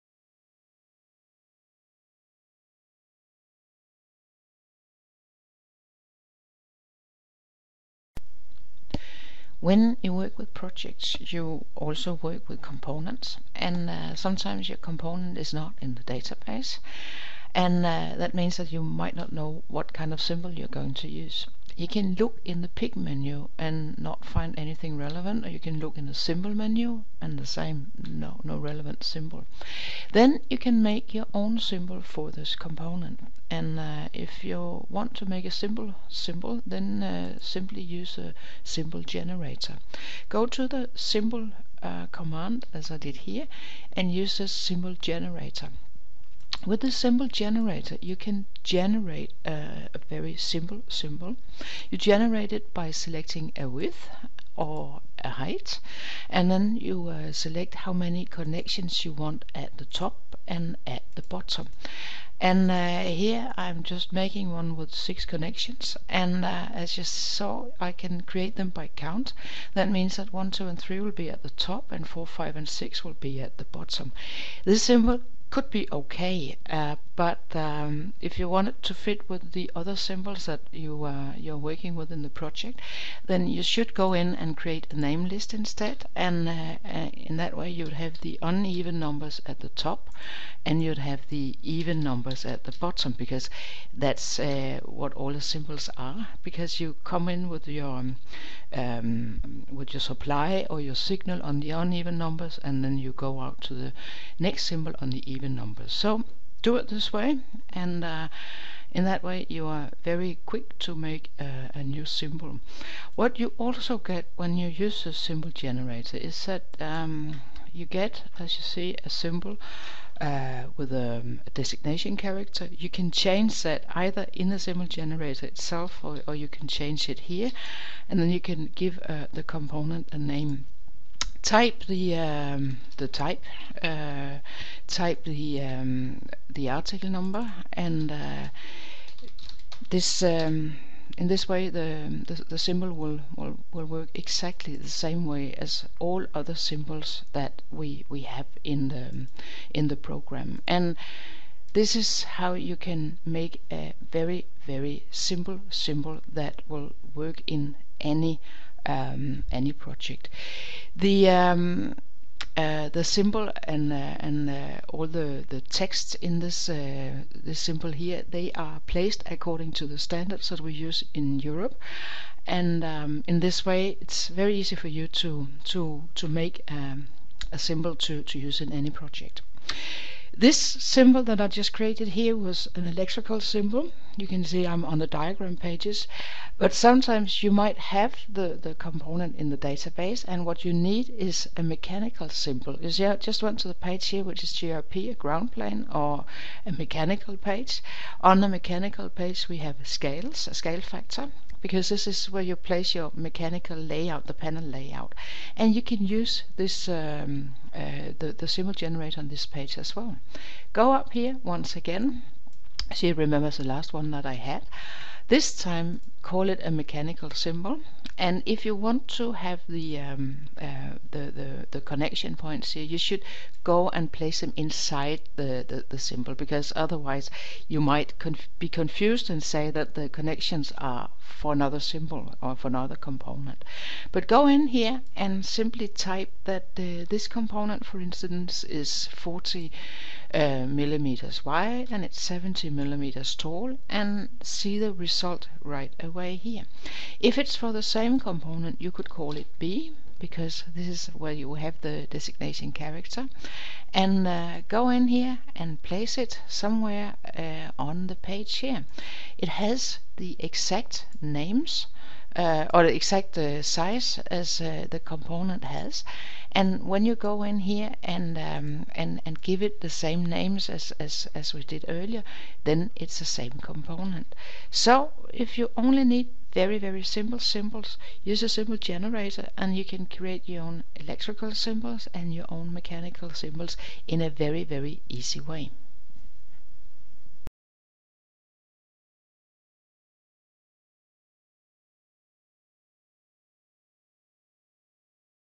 Create your own symbol: Create a simple symbol Part 1 (1/10)
See how to use the Symbol Generator in PCSCHEMATIC Automation to create simple symbols for electrical diagrams and for mechanical layouts.